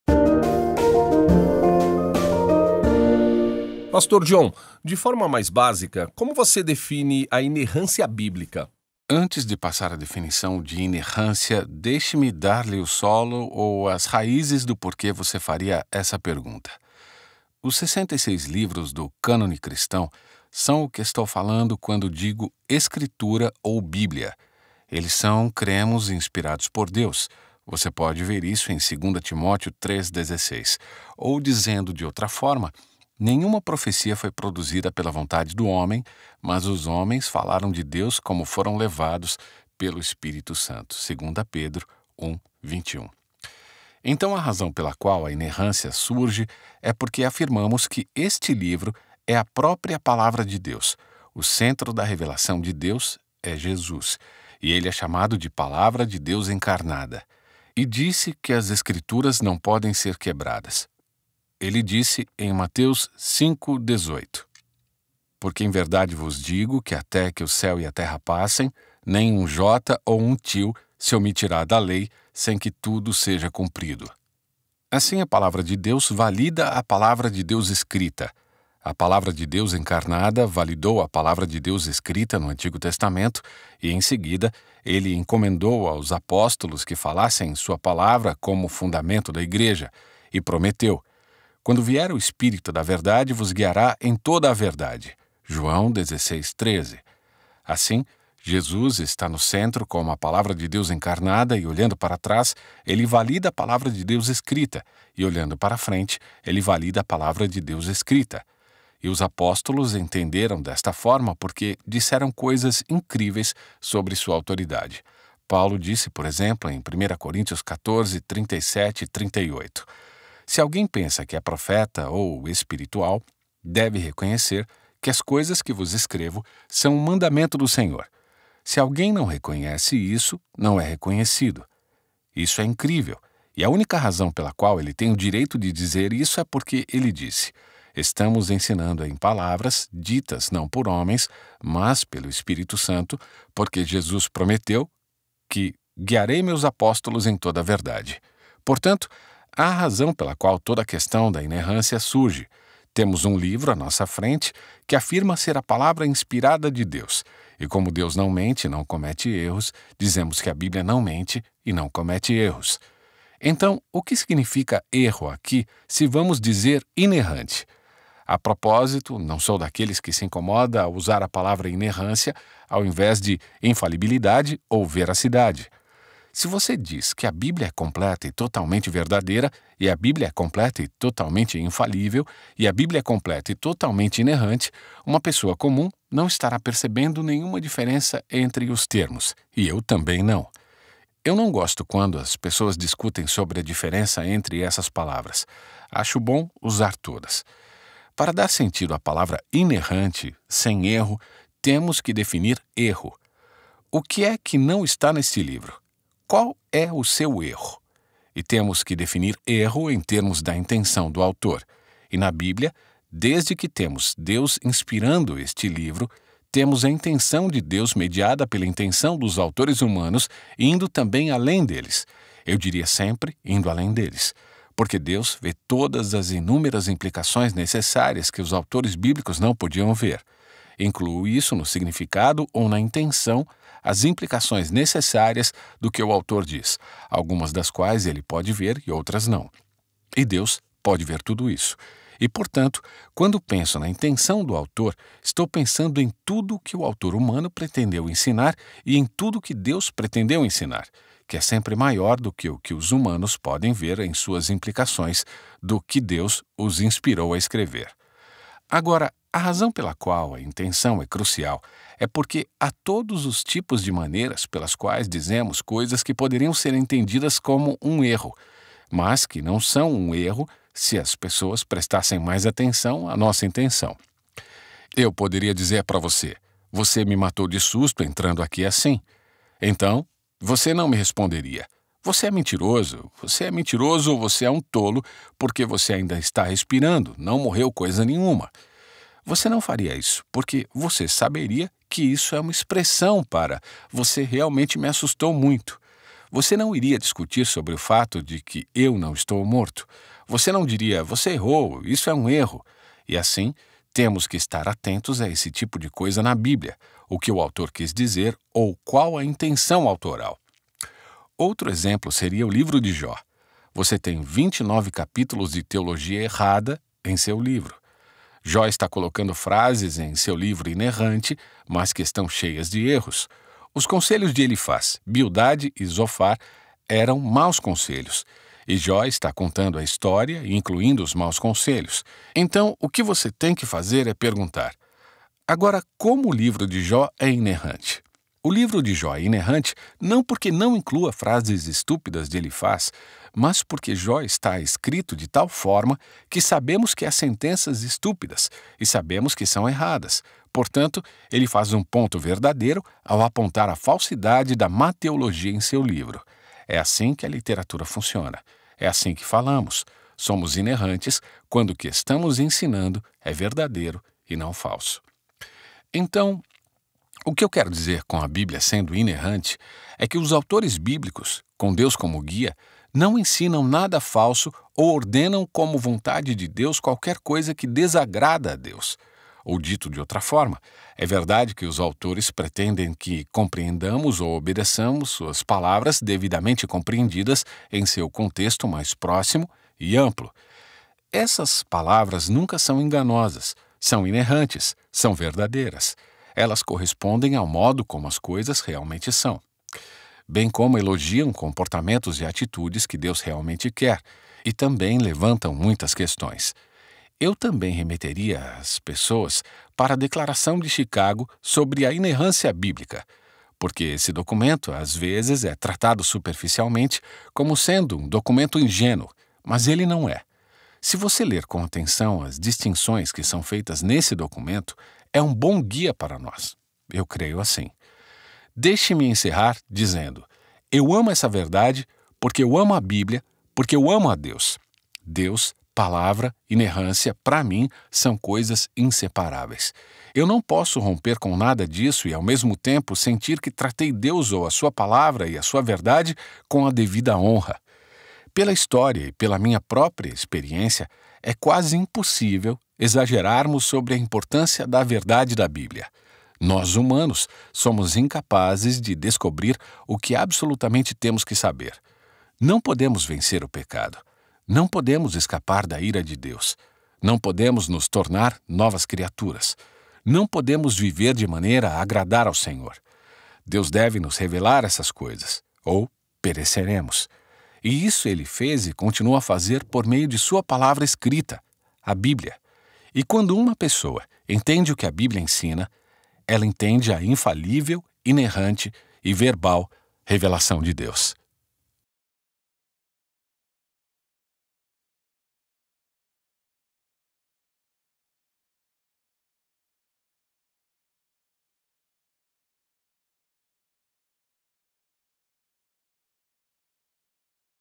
O que significa inerrância bíblica? // John Piper Responde
John Piper Responde neste episódio o que é inerrância bíblica e como compreendê-la é importante para a nossa vida com Cristo.

Traduzido com permissão de @desiringGod.

Versão Brasileira:
Estúdio: DBvoz Studios
Tradução: Arthur Baguera
Revisão: Vinicius Lima
Edição: Eduardo Costa
Mixagem: DBVoz Studios
Editor gerente do projeto: Vinicius Lima

Com vozes de
John Piper: Duda Baguera
Entrevistador: Márcio Cruz

VOLTEMOS AO EVANGELHO